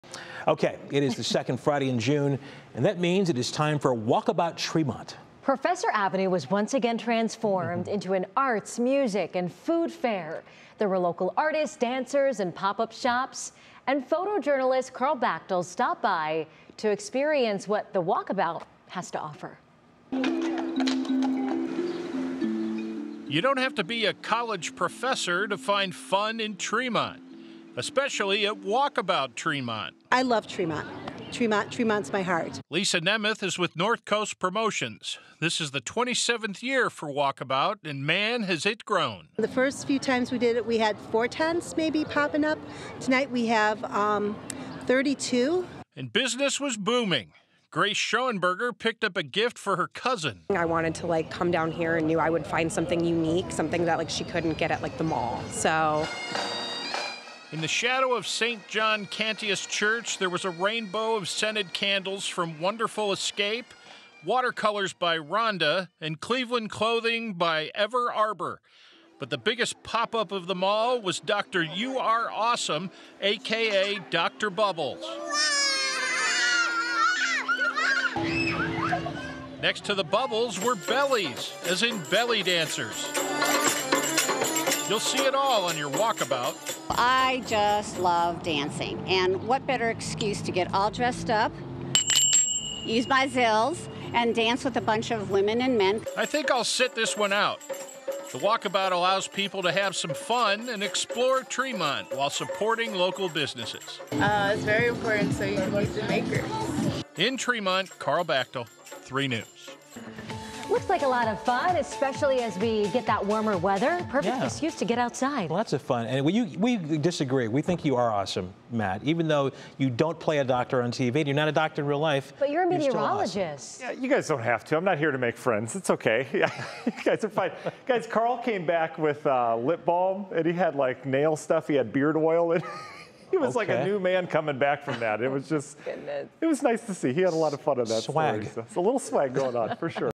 Walkabout Tremont shows off one of Cleveland's oldest neighborhoods
The second Friday in June means it's time for Walkabout Tremont. Professor Avenue was once again transformed into an arts, music, and food fair. There were local artists, dancers, and pop-up shops.

You don't have to be a college professor (or even live on College Ave. or Professor Ave.) to find fun in Tremont. Especially at Walkabout Tremont.

Lisa Nemeth is with Northcoast Promotions. This is the 27th year for Walkabout and man, has it grown!

"The first few times we did it, we had like 4 tents popping up," recalls Nemeth. "Tonight (Friday) we have 32, so this is probably the largest pop-up that we've had."

And business was booming. Grace Shoenberger picked up a gift for her cousin. "I wanted to come down here and I knew I would find something unique, like something she like couldn't get at like the mall," Grace tells us.

In the shadow of St John Cantius Church, there was a rainbow of scented candles from Wonderful Escape, watercolors by Rhonda and Cleveland clothing from Everarbor. But the biggest pop-up of them all was Dr. U R Awesome, AKA Dr. Bubbles. Next to the bubbles were bellies. As in belly dancers.